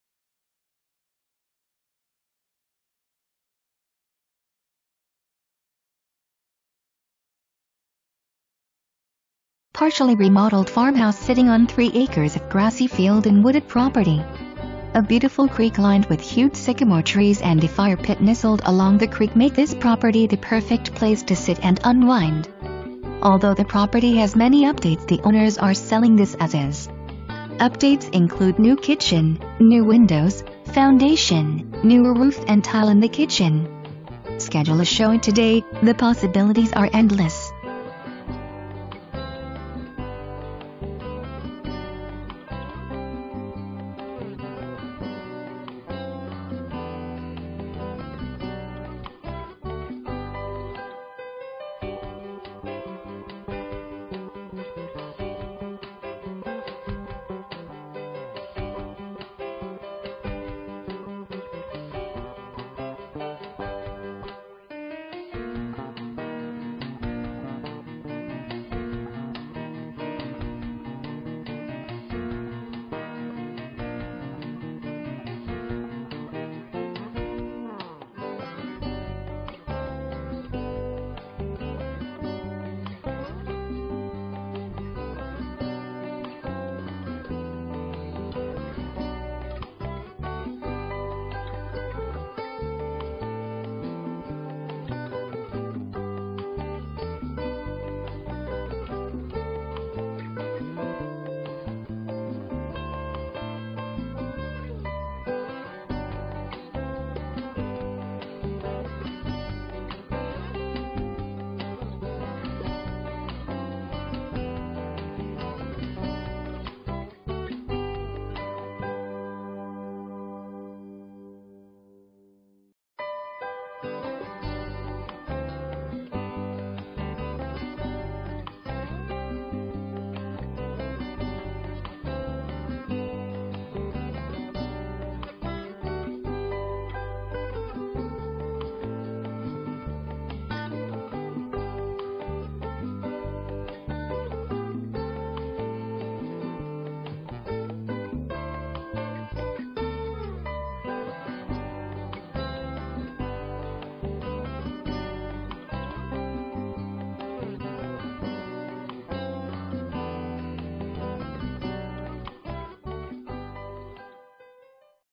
Homes for Sale - 7693 Bob Allen Road, Freetown, IN
7693 Bob Allen Road
Freetown, IN 47235

Partially remodeled farmhouse sitting on 3 acres of grassy field and wooded property. A beautiful creek lined with huge sycamore trees and a fire pit nestled along the creek make this property the perfect place to sit and unwind! Although the property has many updates the owners are selling this "As Is". Updates include new kitchen, new windows, foundation, newer roof and tile in the kitchen. Schedule a showing today, the possibilities are endless!!!

Presented By:
Penny Scroggins, Carpenter Realtors